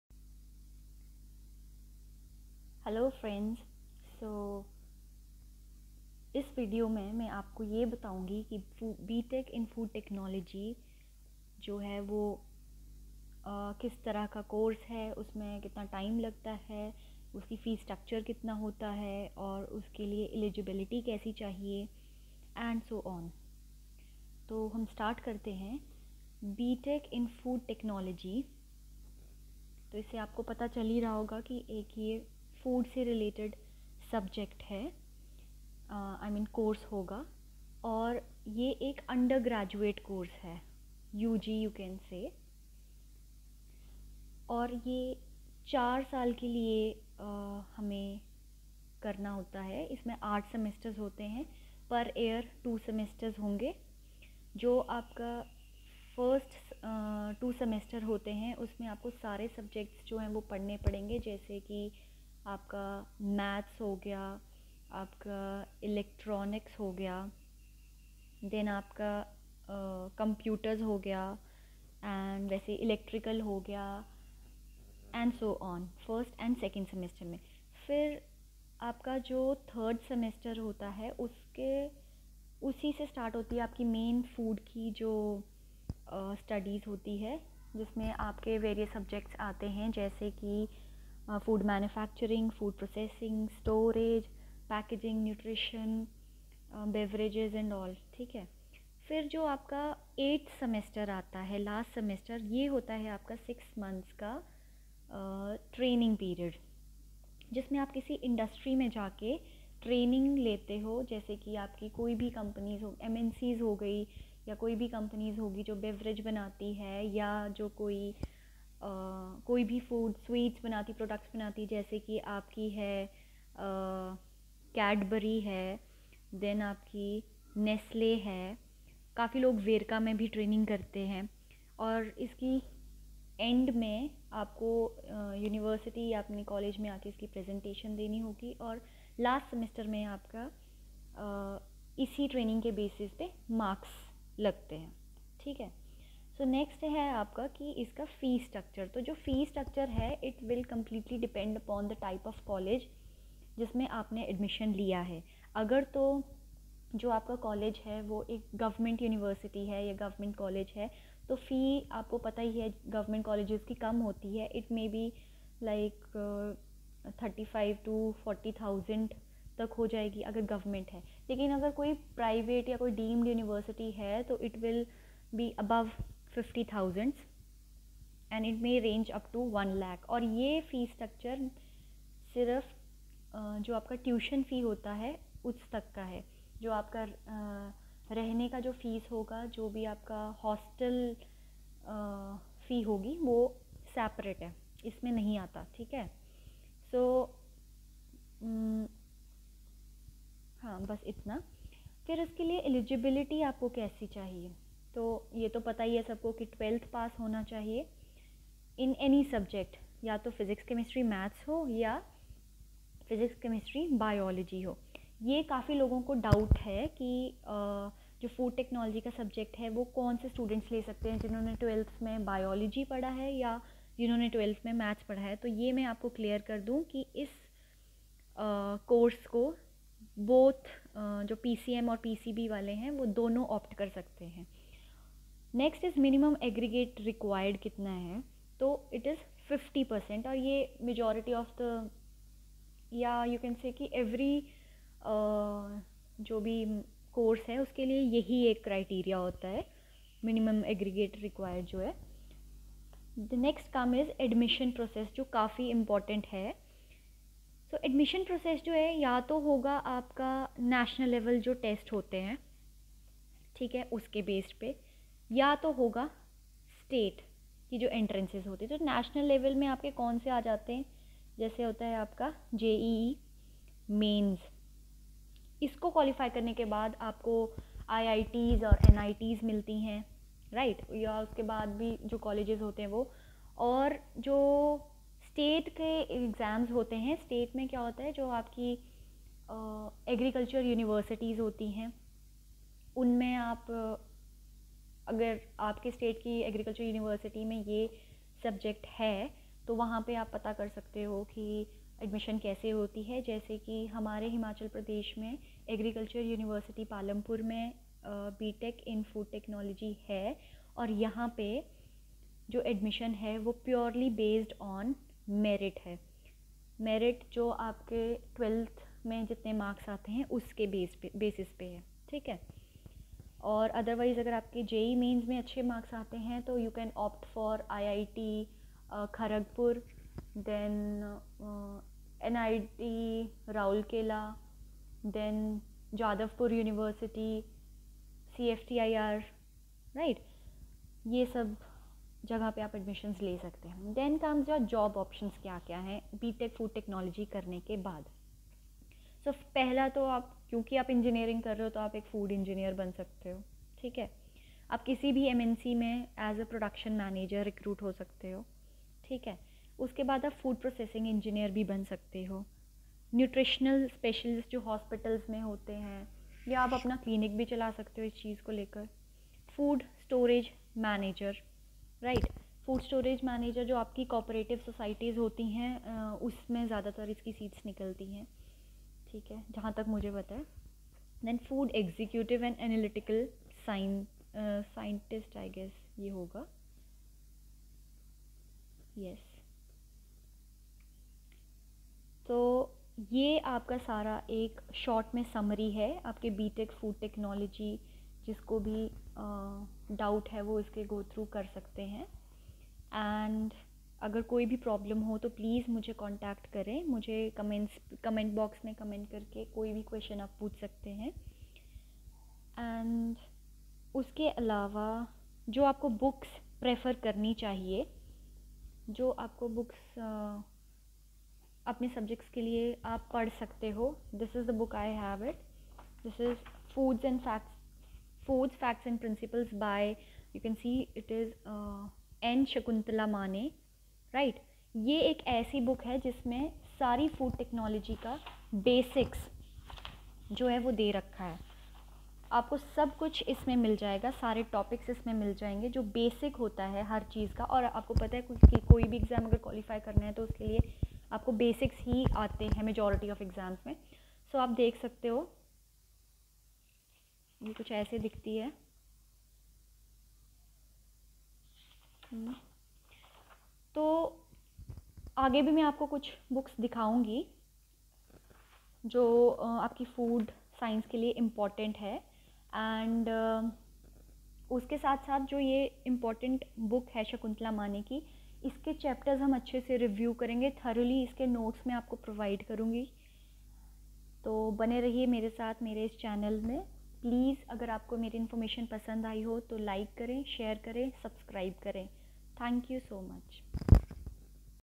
B.Tech Food Technology | course details |eligibility | fee structure| Job options
In this vedio you can see the course detail of B.Tech in Food Technology, which includes its level, duration, fee structure, eligibility,  minimum aggregate required,  admission process and job options.In the next vedio I will discuss about ICAR NET 2021 exam.